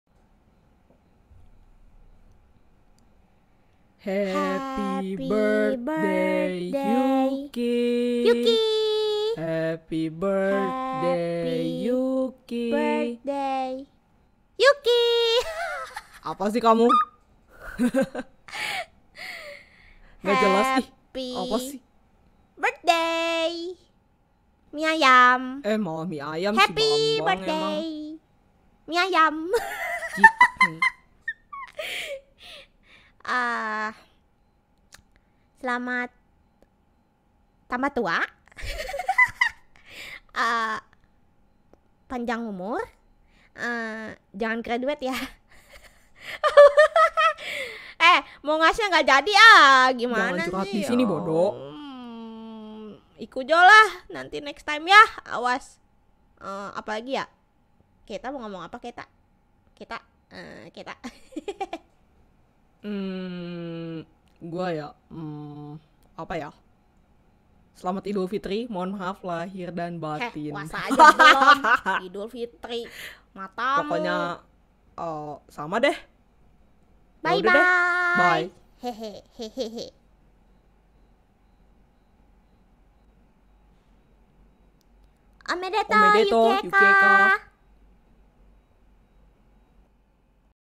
Raw Video Ucapan Ultah Untuk Yuki Eka 【Dibuang Sayang #1】
Sampurasun~ Dibuang sayang~
Kemarin eror katanya, backgroundnya gak bisa diganti jadi di projek video ultah Yuki Eka kemarin Keikonya jadi PNG lagi hiks ;w; mengsedih ;w;

[HASHTAG]
Voice Acting